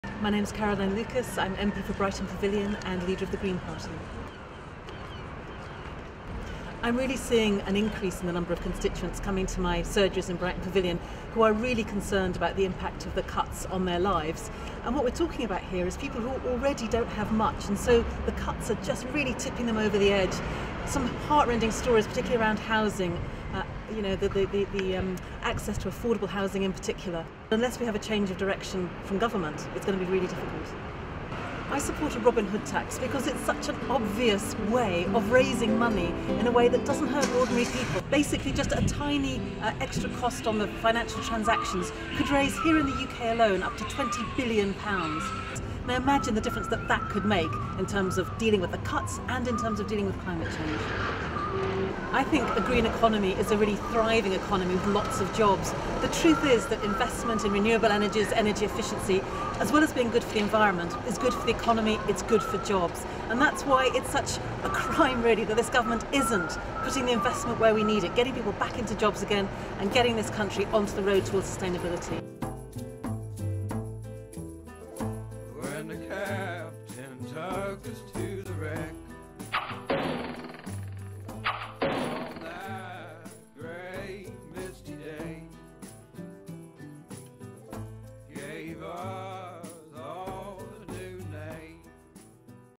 Robin On The Road Meets Caroline Lucas
We met Caroline Lucas in Brighton to talk positive alternatives to the cuts, the Robin Hood Tax and what a green economy could look like.